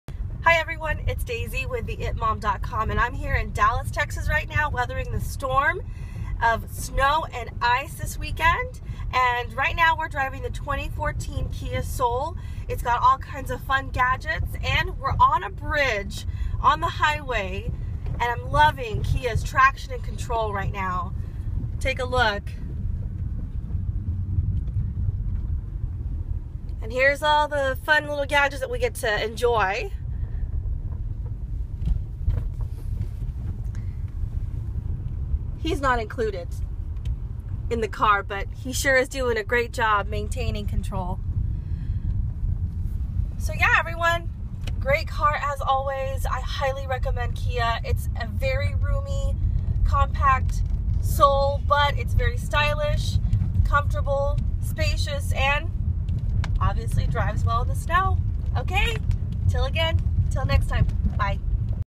2014 Kia Soul Mommy Blogger Review
| We drove the 2014 Kia soul in the snow and ice here in Dallas, Texas.